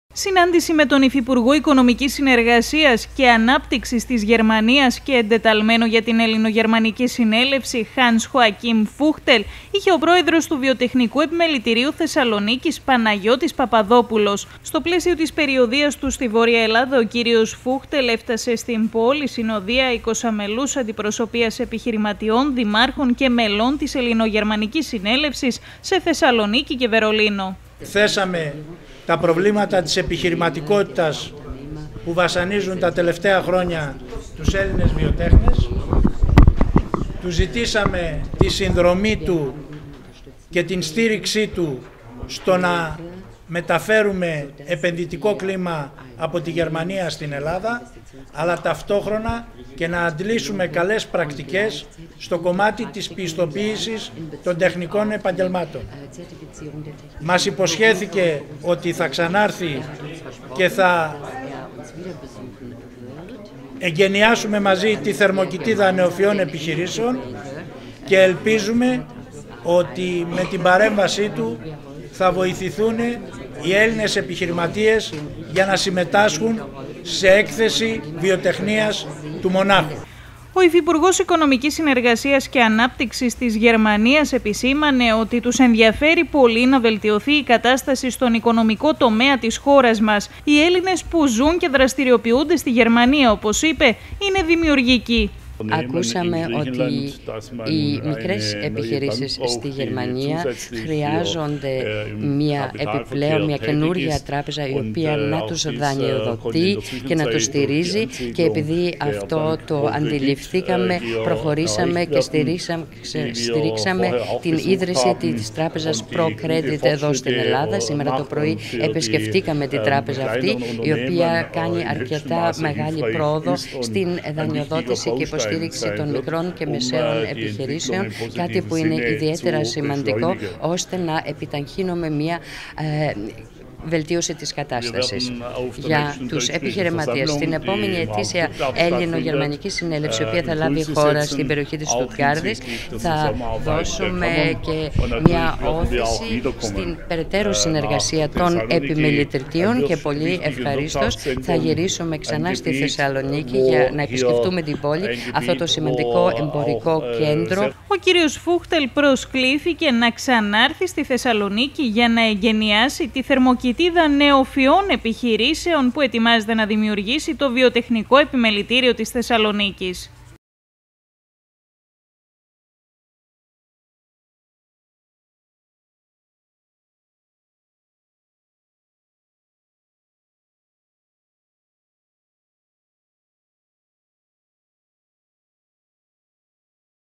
ΣΤΗ ΘΕΣΣΑΛΟΝΙΚΗ Ο ΦΟΥΧΤΕΛ
Το Βιοτεχνικό Επιμελητήριο Θεσσαλονίκης επισκέφθηκε ο Γερμανός Υφυπουργός. Η κάμερα του Atlas TV ήταν εκεί...